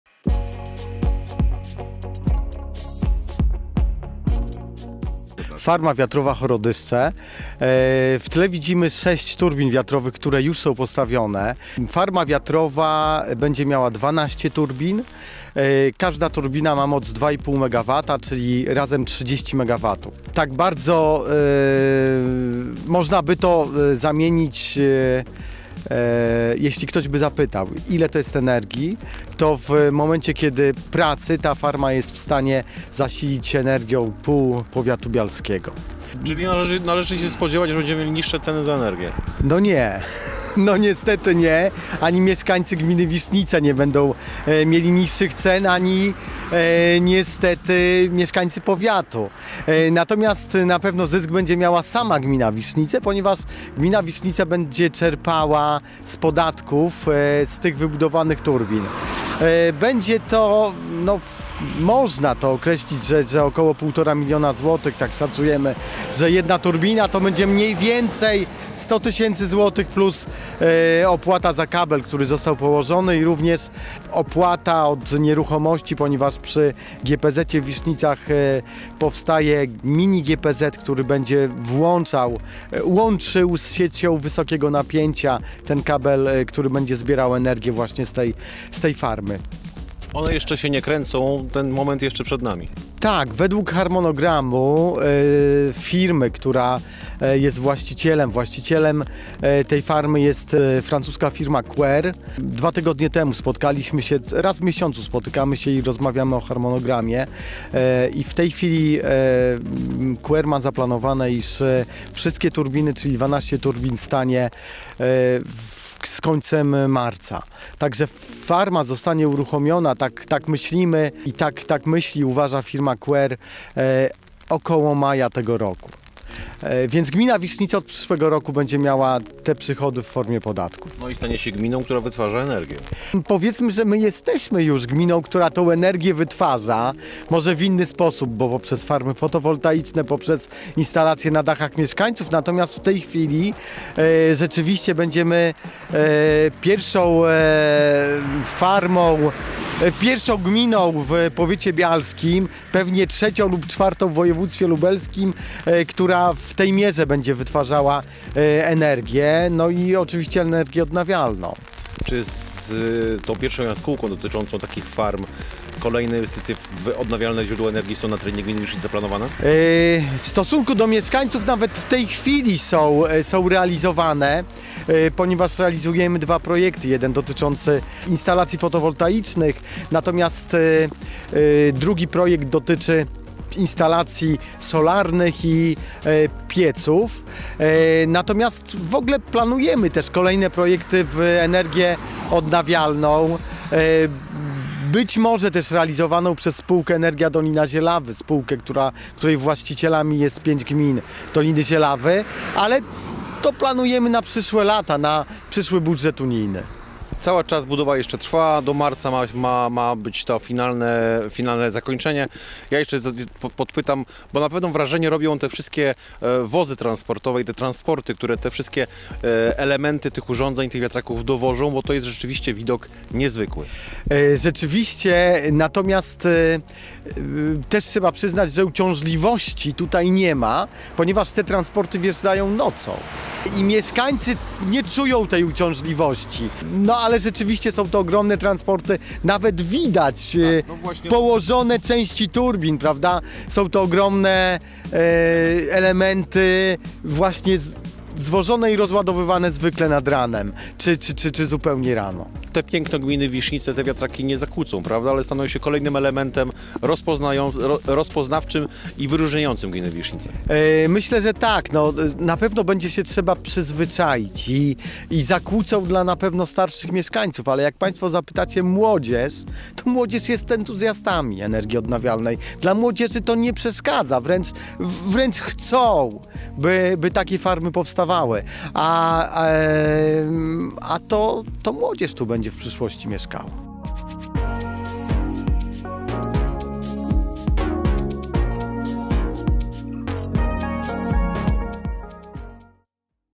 Jest już 6. z 12. ! Powstaje Farma Wiatrowa w Gminie Wisznice
Piotr Dragan Wójt Gminy Wisznice opowiedział nam o powstającej farmie wiatrowej w Gminie Wisznice.